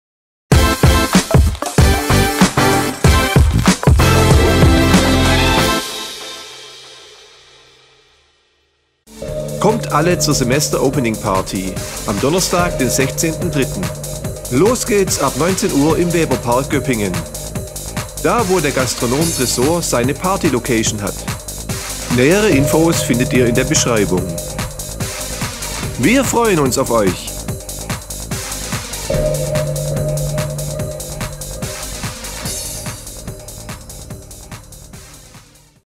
Semester-Opening-Party - Donnerstag, den 16.März !!!
Semester-Opening-Party, die in Kooperation mit dem VWI* und dem Gastronom „Tresor“ aus Göppingen am Donnerstag, den 16.03. ab 19 Uhr im Tresor Weberpark stattfindet.

Dazu sind alle Studierende eingeladen, sowie alle Mitarbeitenden und ProfessorInnen.
Weitere Eckpunkte:
• Beginn: 19 Uhr
• Eintritt: Studierende 3 €, alle anderen: 5 €
• Vergünstigte Getränkepreisliste
• Ort: Weberpark, Heininger Str. 38, GP (das ist in dem neuen Dehner-Gartencenter-Gebäude beim Möbel Rieger in der Nähe, direkt am Kreisel)